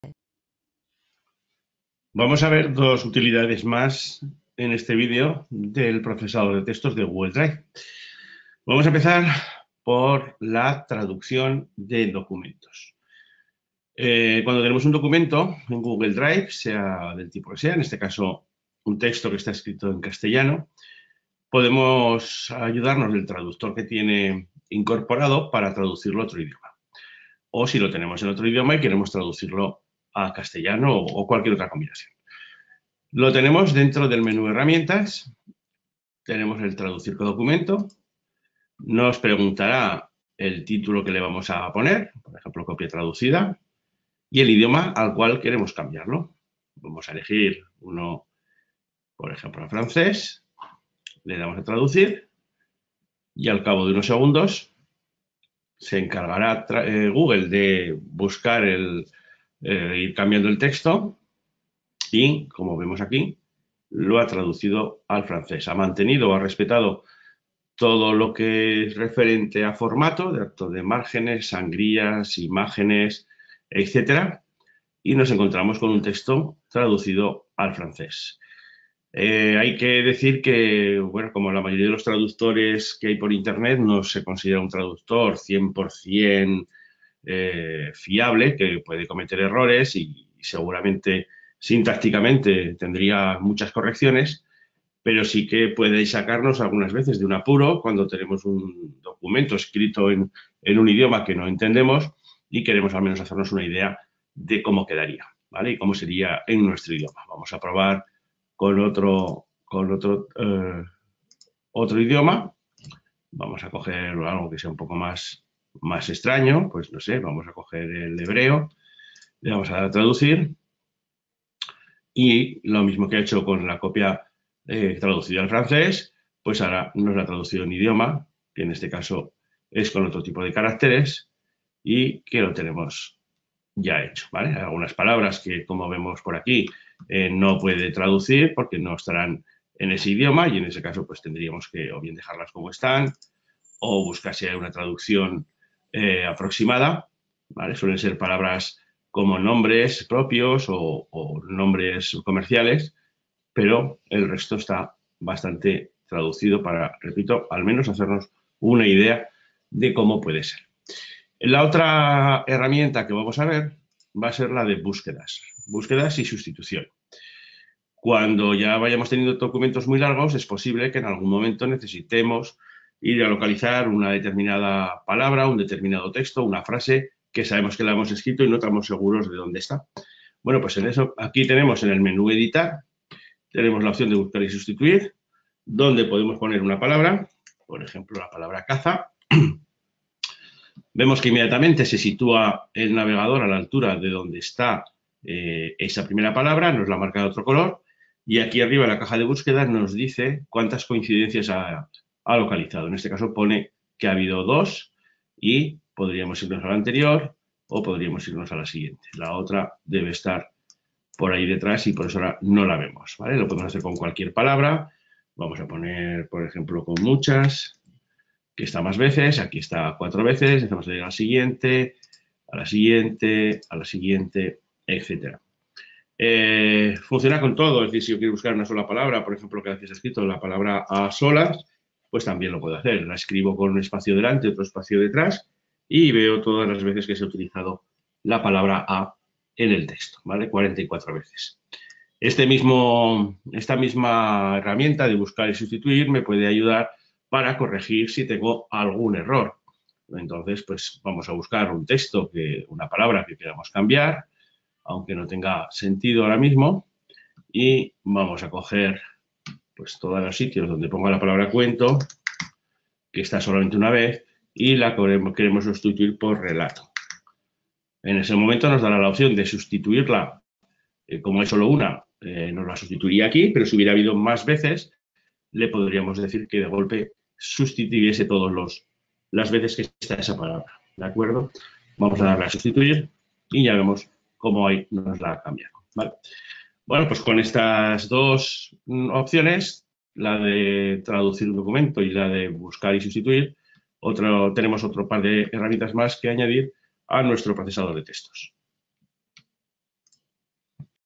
Curso Google Drive básico (17). El procesador de textos 13 traduccion buscar sustituir
Curso Google Drive básico. Inicio de Google drive. Con este curso aprenderás a usar Google Drive. Desde la configuración básica, a compartir información, procesador de textos, hojas de cálculo, presentaciones y formularios.